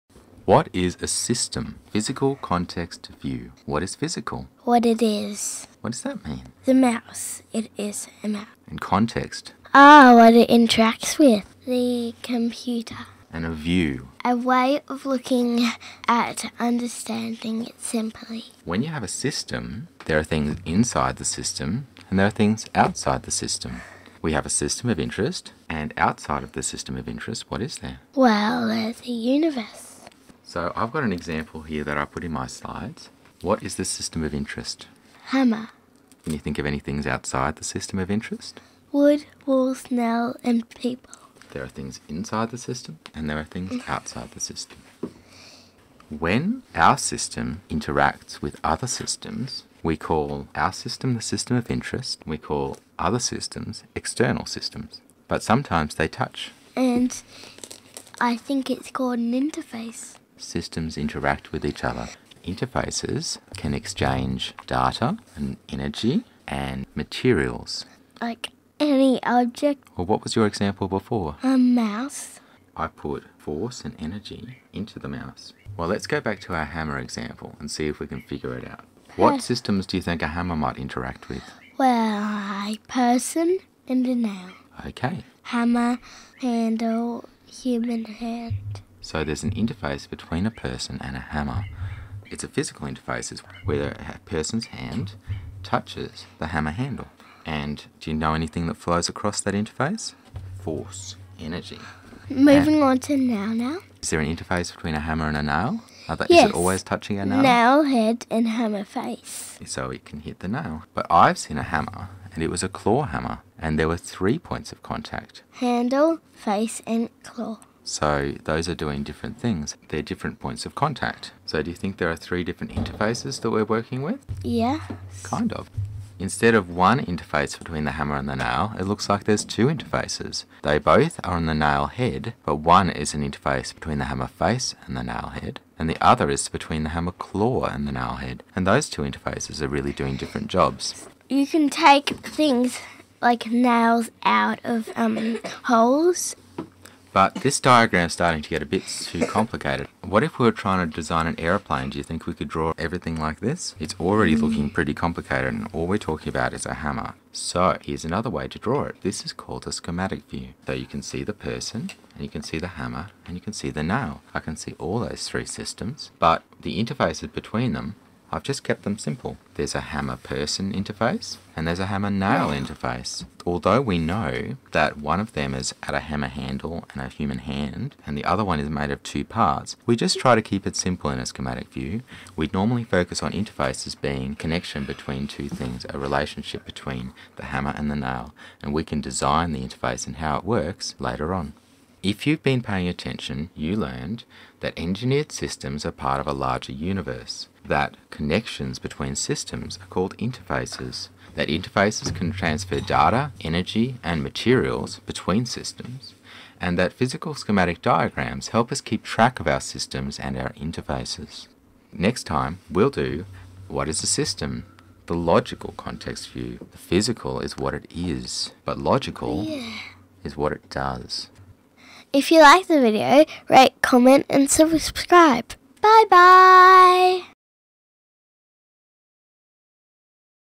Physical Context View (What is a System?)
Miss 7 and I introduce concepts of systems, interfaces, and schematic diagrams from a physical context perspective.